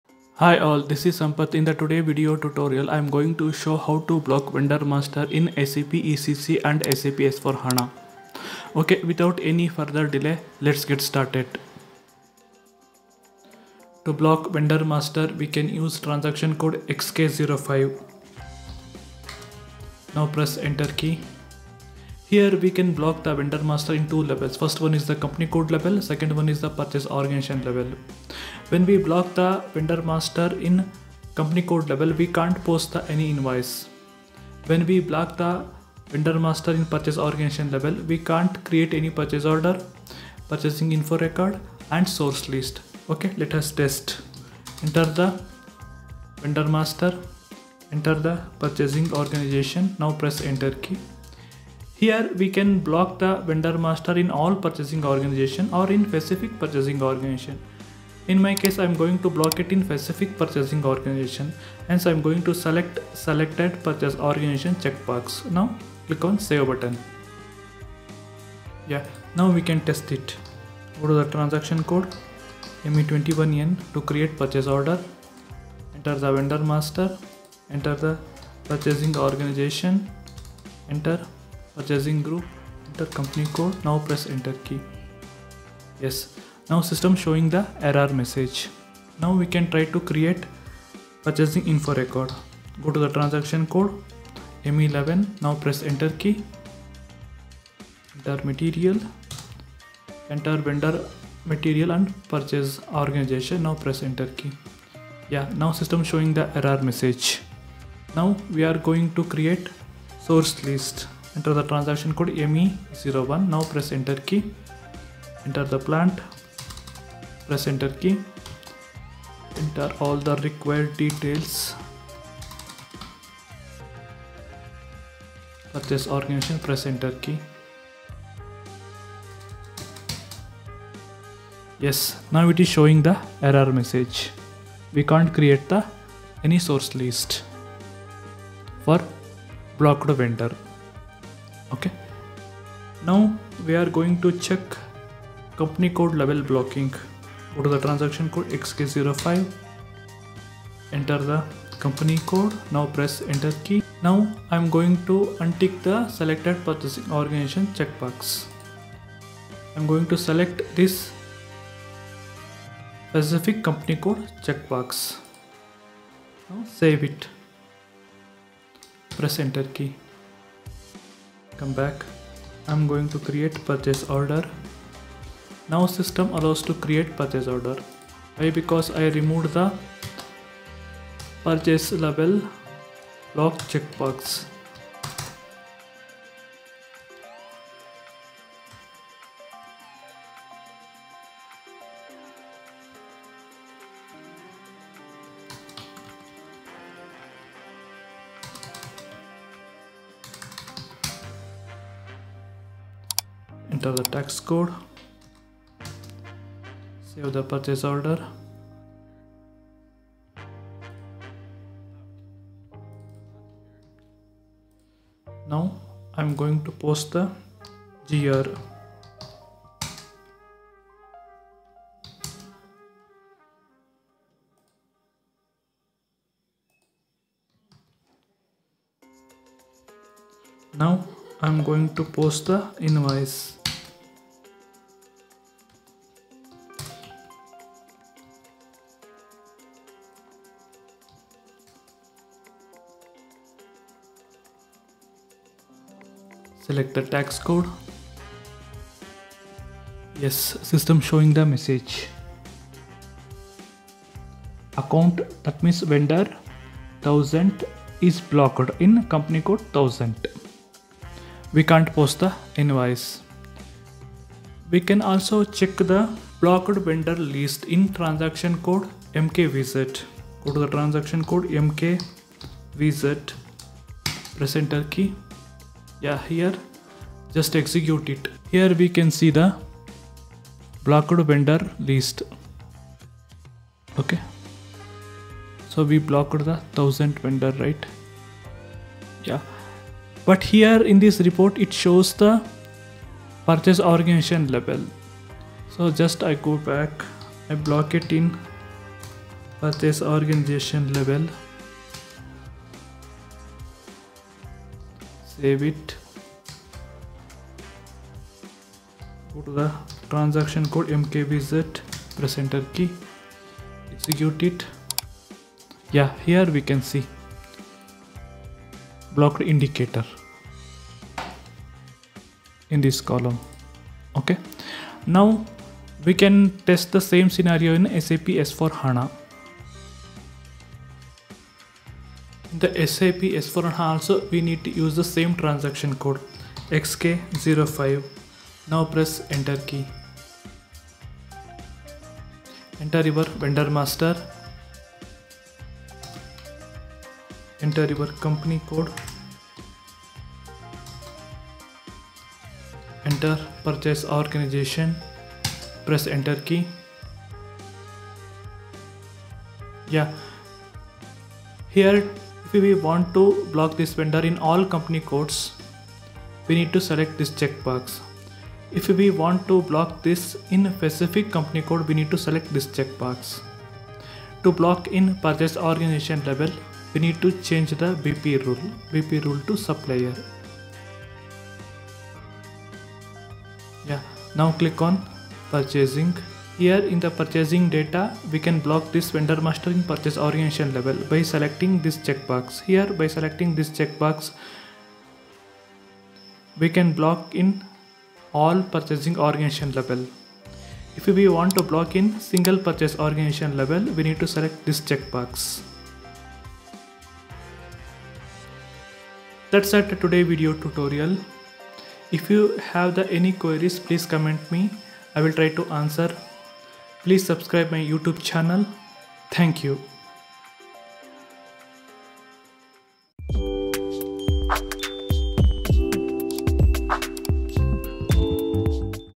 How to Block Vendor master in SAP ECC and SAP S4HANA
In this video, I will show you how to block a vendor master in SAP ECC and SAP S4HANA. Blocking a vendor master prevents it from being used for purchasing, invoicing, and other transactions. This can be useful if you need to temporarily or permanently disable a vendor, or if you need to investigate a vendor for fraud or other issues.